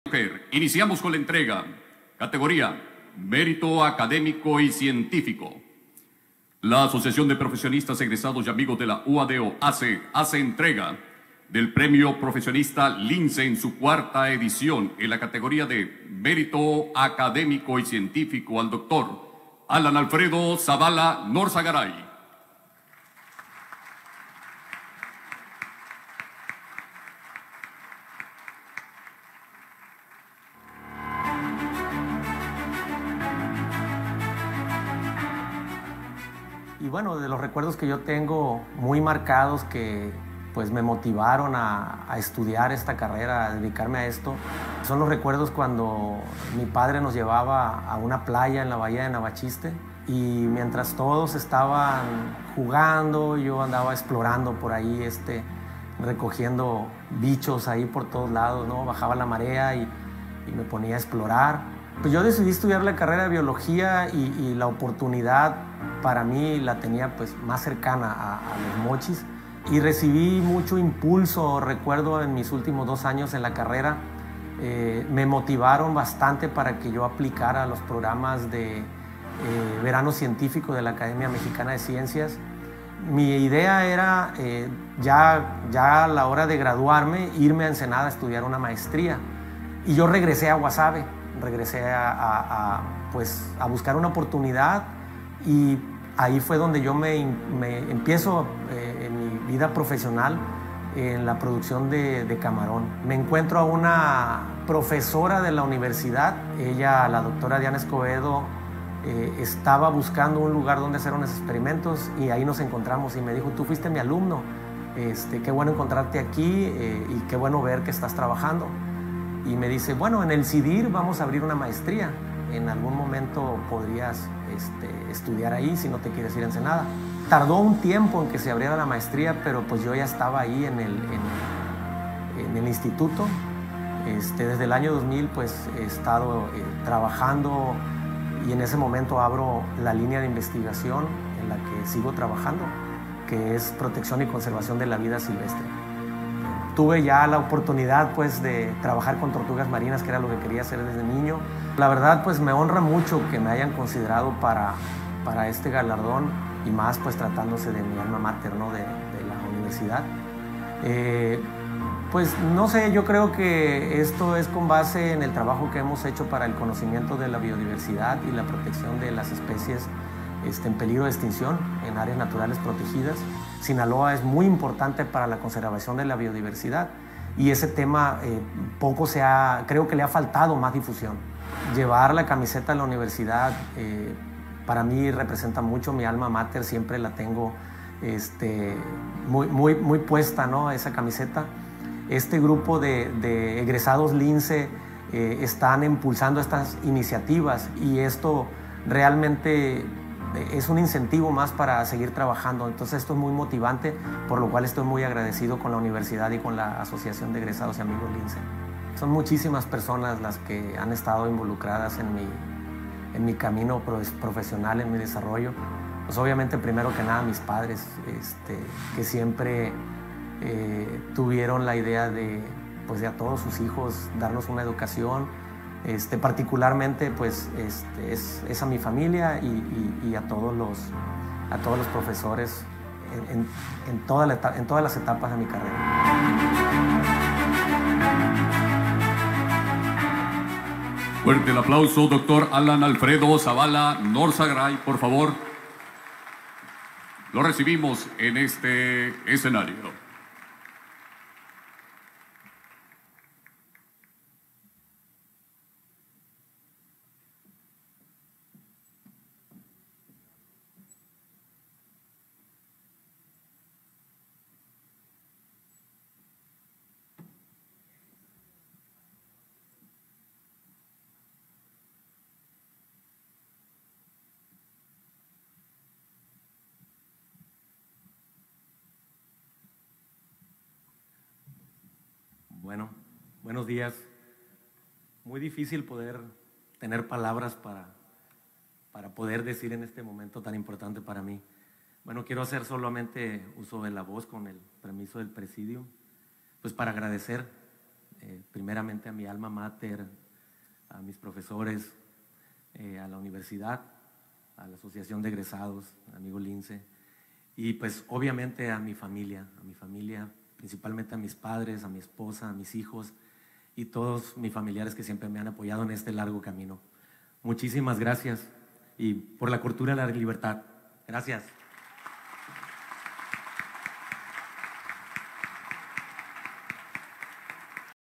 Premio Profesionista Lince 2023 al Dr. Alan Alfredo Zavala Norzagaray
Entrega del Premio Profesionista Lince 2023 de la Universidad Autónoma de Occidente en su categoría de Mérito Académico y Científico  al Dr. Alan Alfredo Zavala Norzagaray